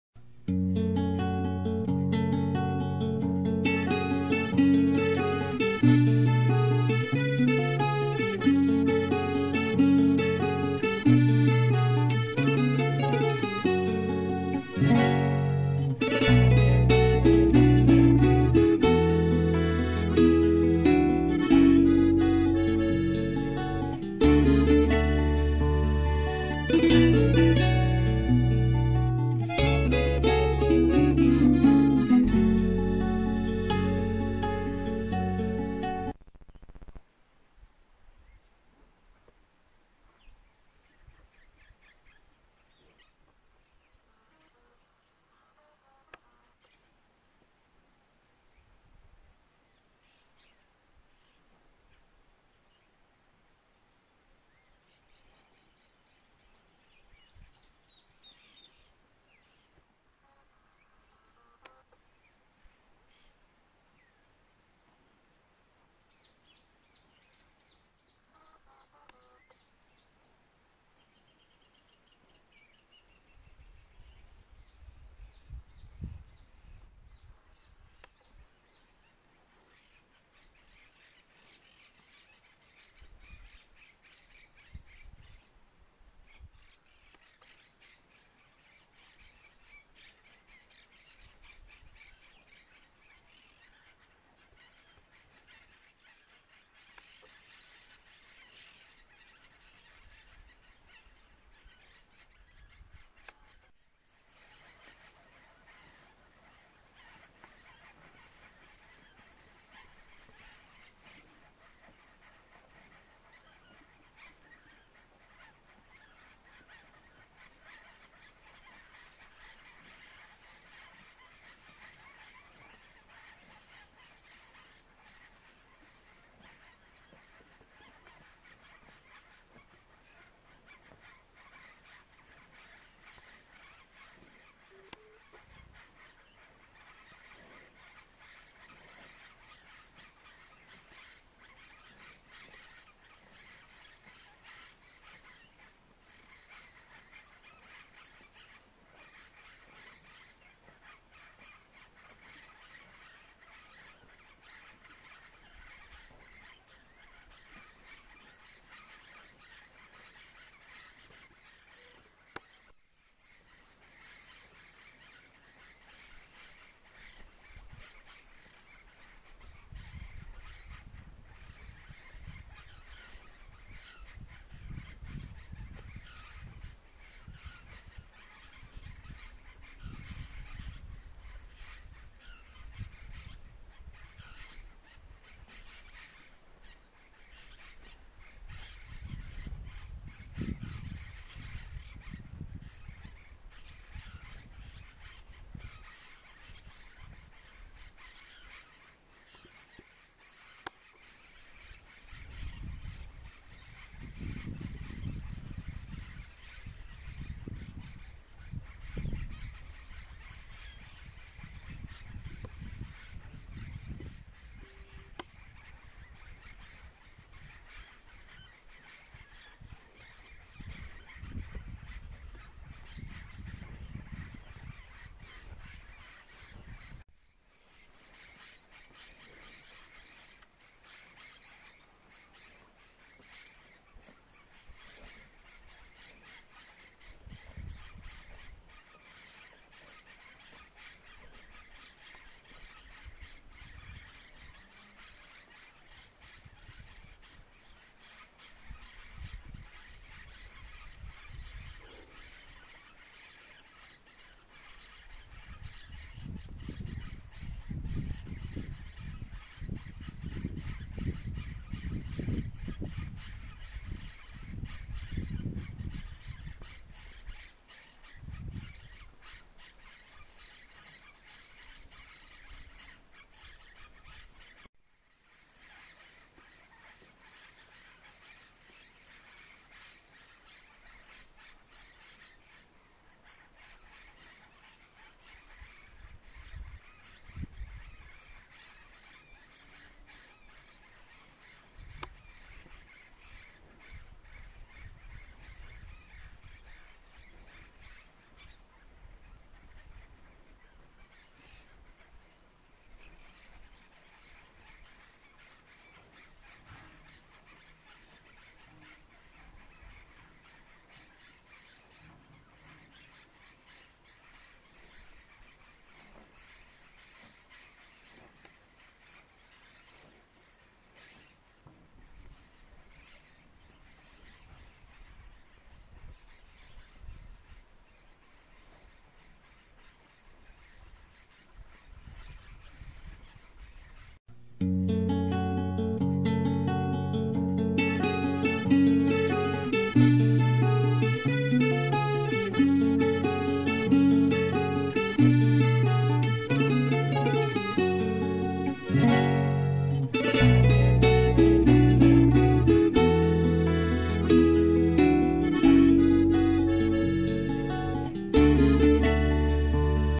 URUBUS E O TERNEIRO DA VACA
Assista o vídeo e contemple essa cena de campo registrada na Fazenda Santa Suzana, em Candiota/RS.